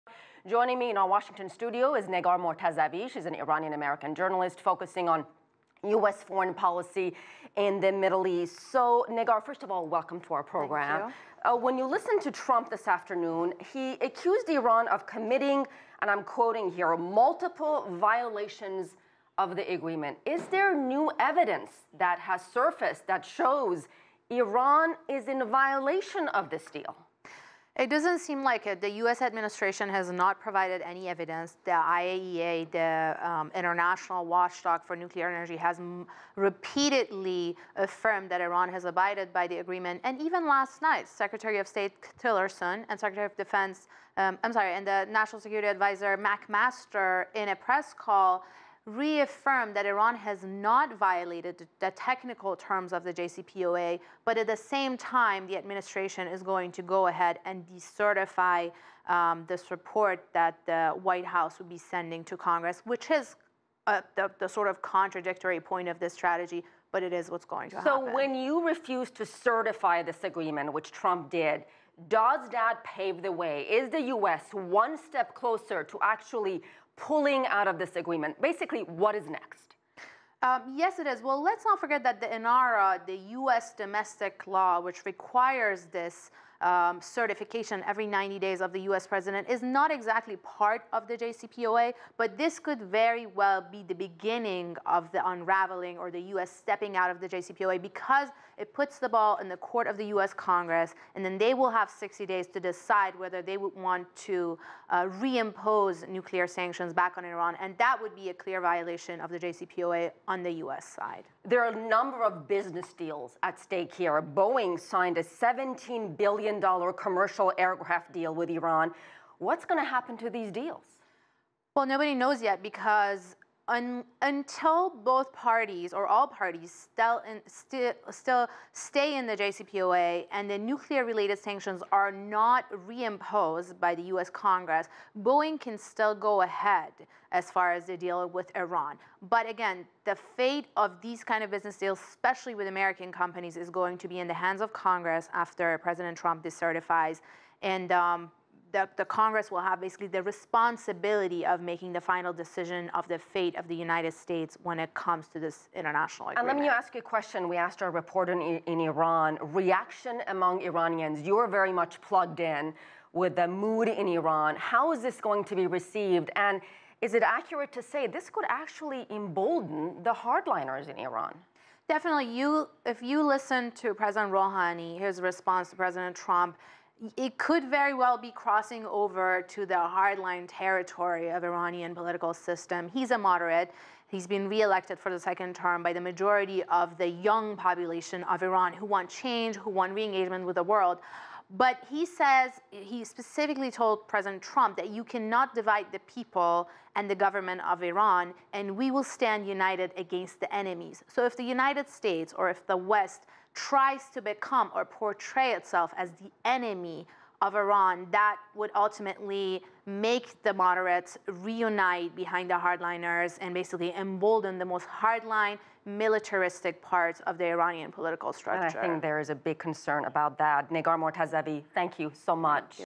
US gives no evidence of Iranian nuclear non-compliance – Negar Mortazavi
U.S. President Donald Trump announced that the U.S. will not certify Iran’s compliance with the 2015 multi-lateral nuclear deal. The future of the agreement is now in question, with Trump giving the American Congress 60 days to decide on the imposition of sanctions against Iran. Negar Mortazavi, an Iranian-American journalist and commentator on American foreign policy in the Middle East, discussed this with CGTN’s Asieh Namdar.